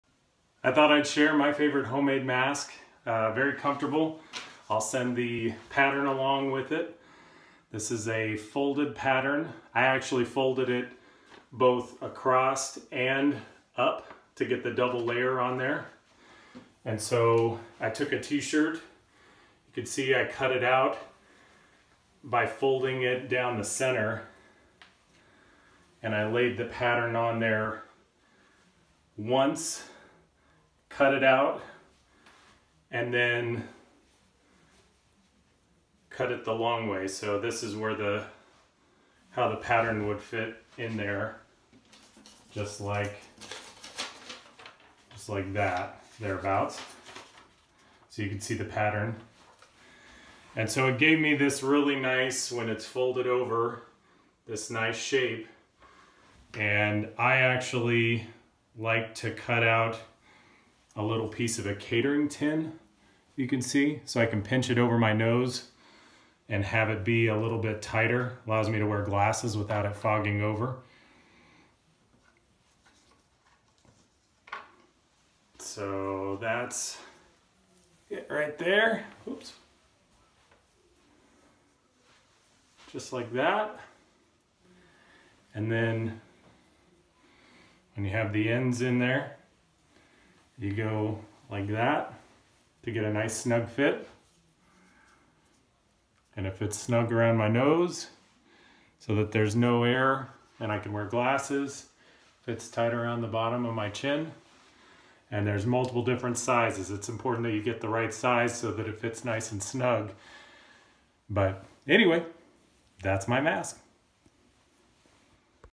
My favorite no-sew homemade mask
😷 My favorite no-sew mask 😷
Very simple to make and super comfortable.